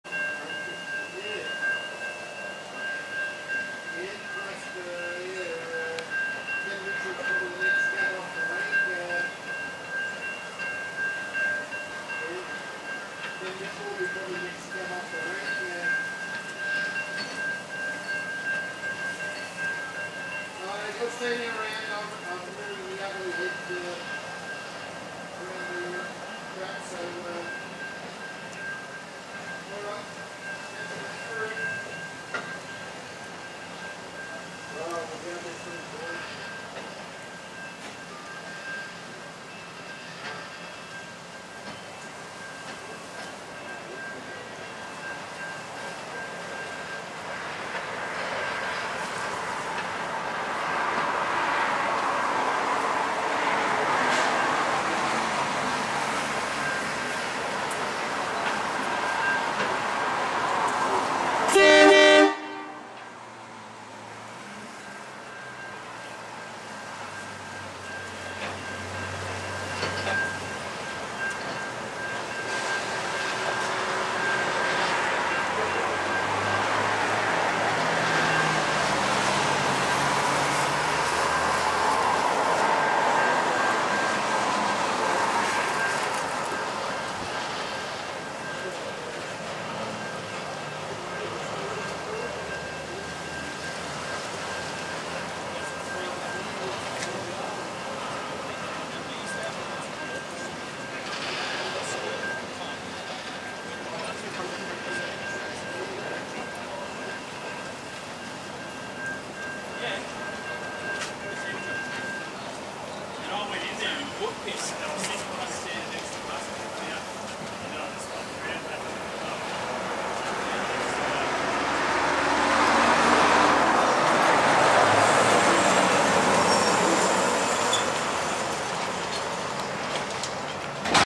47 CLASS COOTAMUNDRA
4708+4702+Carriages reversing into yard at Cootamundra.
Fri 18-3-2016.